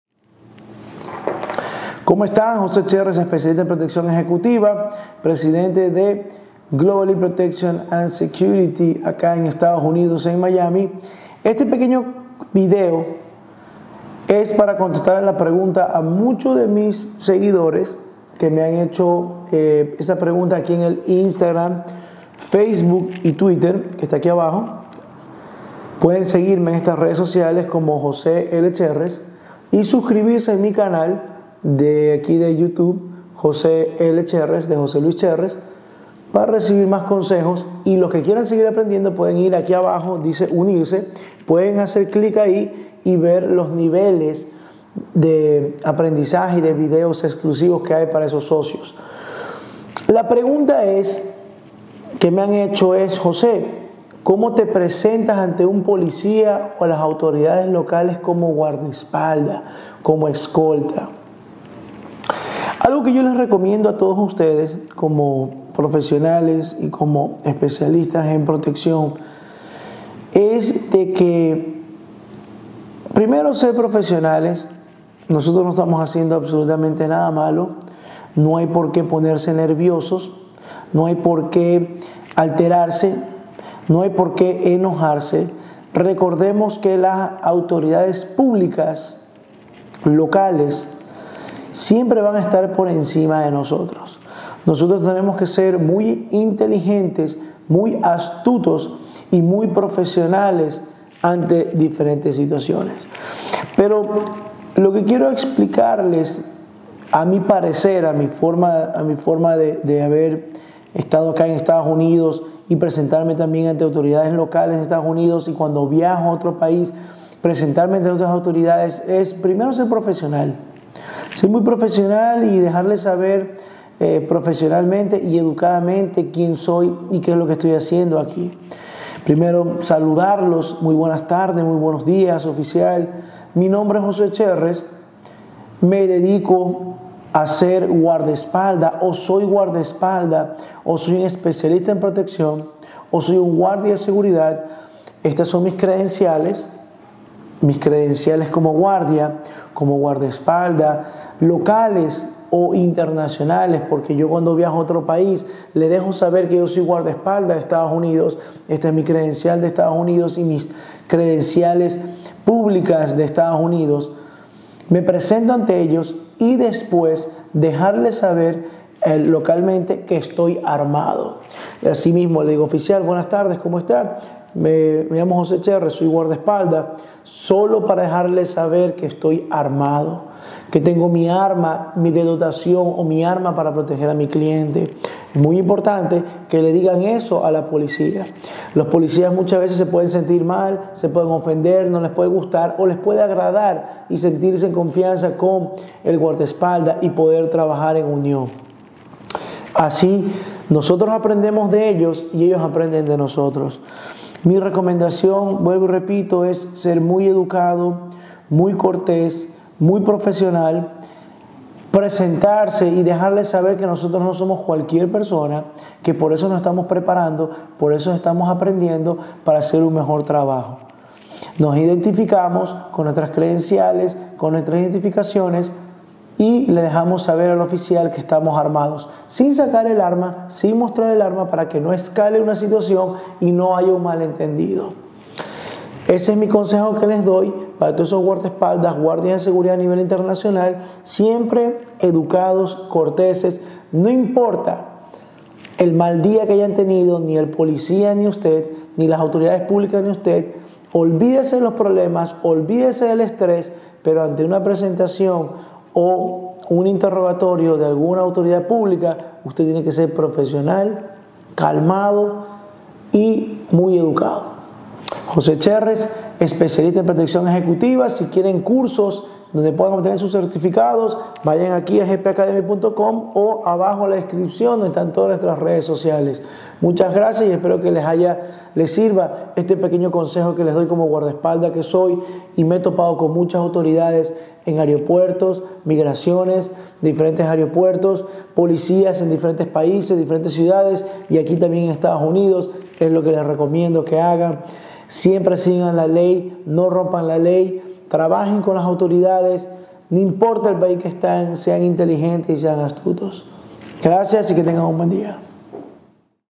Como identificarse ante un Policia
Jose L Cherrez, Especialista en Proteccion Ejecutiva, Instructor Internacional y Empresario les explica como él se identifica ante las autoridades, policías locales e internacionales como Guardaespaldas.

Para ver videos exclusivos y privados con mas temas regístrate en nuestras Membresía en Youtube:

Ropa y accesorios los pueden buscar en el siguiente enlace: